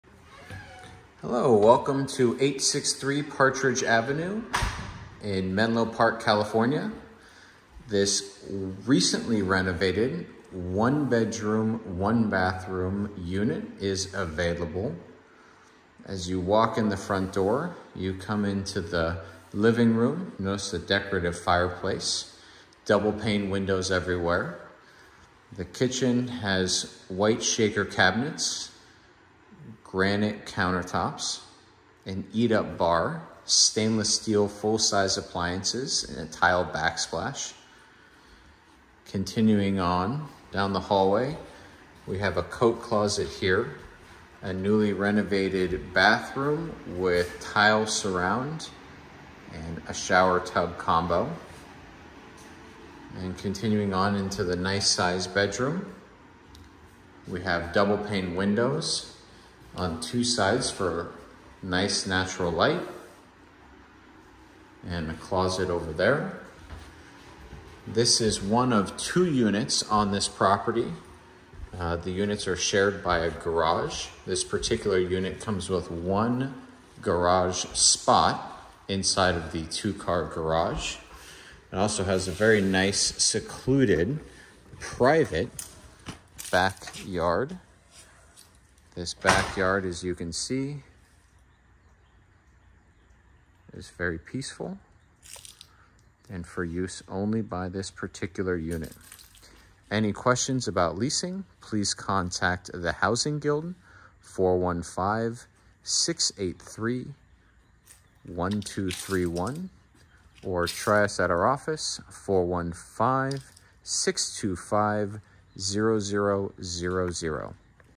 863 Partridge Ave., Menlo Park, CA Housing Guild Virtual Tour 10-10-23
Virtual Tour of a 1br/1ba duplex located at 863 Partridge Ave., Menlo Park, CA.
Secluded Modern Sophistication: Luxe Living in a Renovated Duplex Unit!

Indulge in the epitome of sophisticated living with this exquisite one-bedroom apartment that seamlessly combines modern elegance with classic charm. Every detail of this residence has been carefully curated to offer you a lifestyle of comfort and convenience.

The heart of the home, the renovated kitchen, beckons with an eat-up bar featuring a quartz countertop, stainless steel full-size appliances, a dishwasher, and a disposal. The open concept floor plan creates a fluid connection between the kitchen and living spaces, where a decorative fireplace adds a touch of warmth and character.

Luxuriate in the renovated bathroom, complete with a tile shower/tub surround, offering a spa-like retreat within the comforts of your own home. Throughout the apartment, engineered hardwood floors exude a timeless appeal, while double pane windows provide both insulation and tranquility.

For added convenience, find private laundry machines within the unit and revel in the luxury of a garaged parking space, shared with the neighboring unit in this detached duplex. The shared garage provides an additional layer of privacy between the two residences.

Step outside and discover a large, private, and peaceful backyard, offering an ideal escape for relaxation or outdoor gatherings. Whether you're unwinding in your own sanctuary or exploring the vibrant surroundings, this residence offers the perfect balance of tranquility and accessibility.

The location is unbeatable!! Don't miss the opportunity to make this exceptional apartment your new home — where every detail reflects a commitment to quality living. Your oasis in the heart of Menlo Park awaits!

• One bedroom apartment
• Renovated Kitchen
• Eat-up bar quartz counter top
• Stainless steel full size appliances
• Dishwasher and disposal
• Open concept floor plan
• Decorative fireplace
• Forced air heating
• Renovated bathroom w/tile shower/tub surround
• Engineered hardwood floors throughout
• Garaged parking space (shared garage)
• Private laundry machines
• Large, private and peaceful backyard
• Double pane windows
• Detached duplex unit (garage separates two units)

• Excellent location; walking distance to Safeway, Santa Cruz Ave, Stanford Shopping Center. Two blocks from Nealon Park. A long walk or short bike to Stanford Campus, downtown Palo Alto. Easy biking anywhere!
• CalTrain is a ~15 min. walk, very close to many SamTrans stops on El Camino

From Walk Score:
Walk Score: 76 "Very Walkable; Most errands can be accomplished on foot."
Transit Score: 50 "Good Transit; Many nearby public transportation options."
Bike Score: 90 "Biker's Paradise; Daily errands can be accomplished on a bike."

We rent in accordance with the Fair Housing Act and fair housing guidelines. Any staging, whether real or virtual, is for demonstration purposes only. Unit will be delivered vacant.